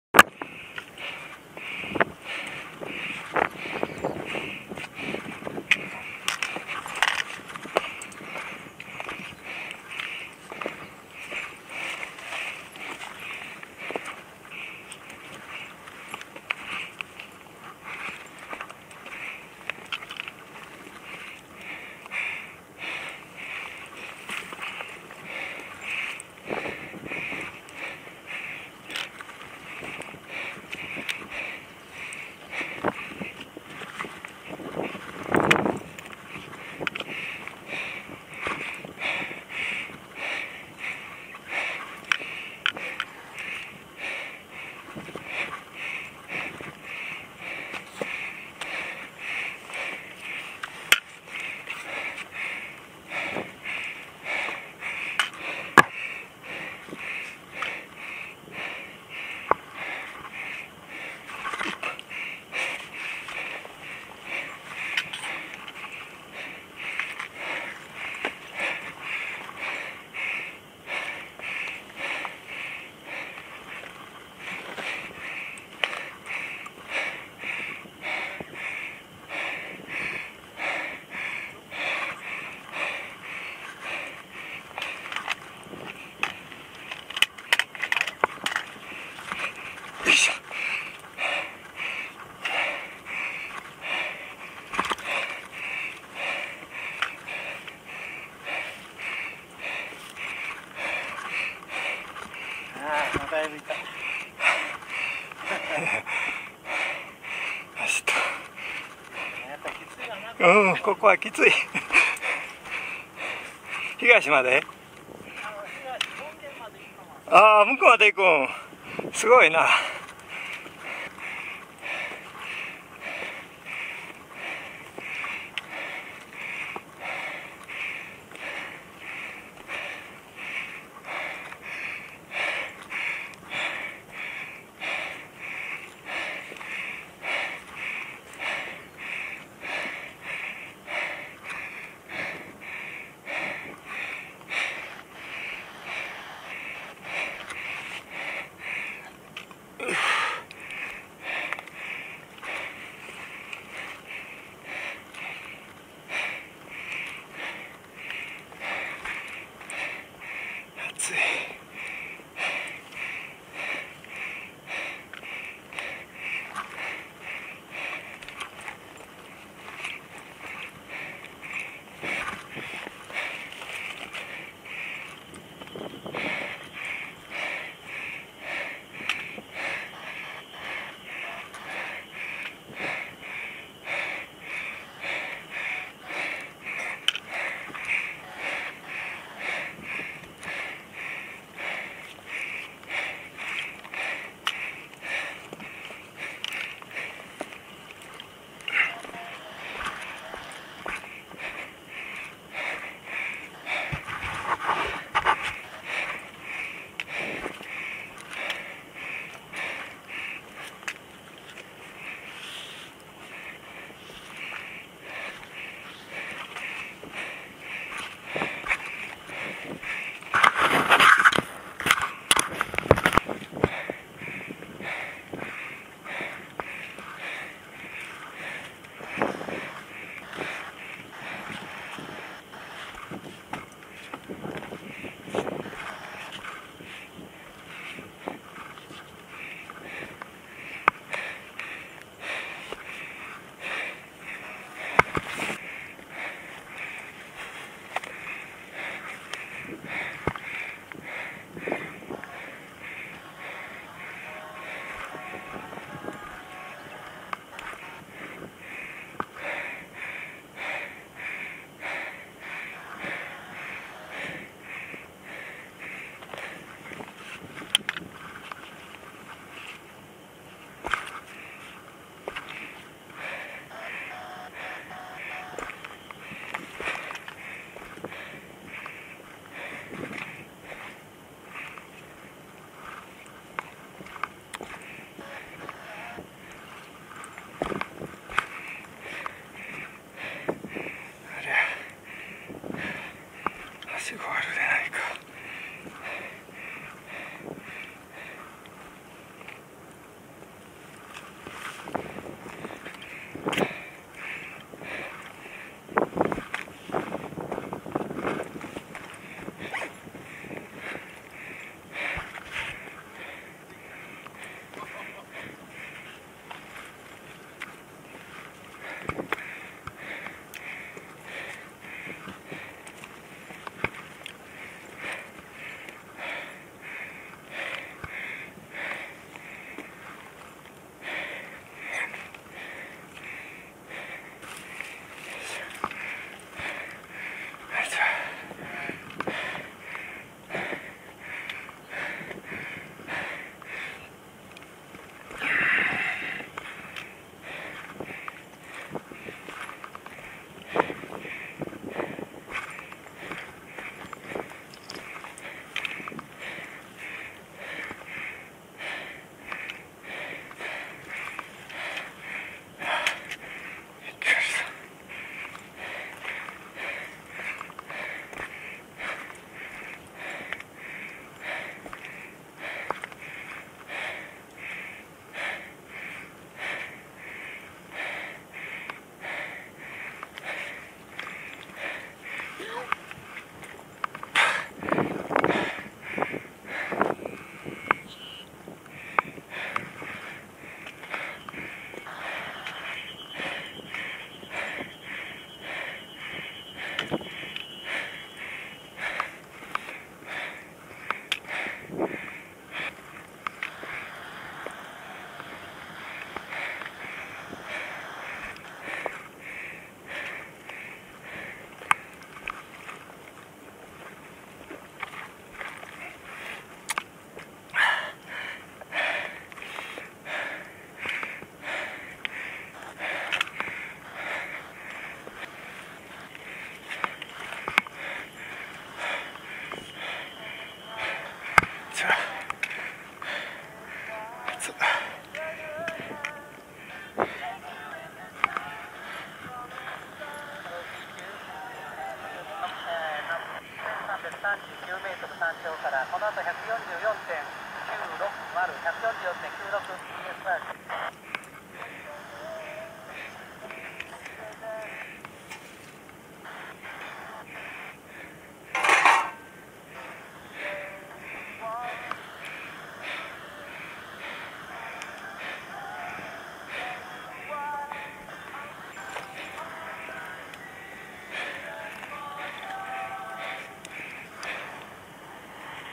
東赤石岩稜帯をあるくPart 2 HX-A100撮影
パナソニックHX-A100で撮りました。
Part1からの続きです。
まだおこしでない方も是非一度おこしください。
いい山ですよ。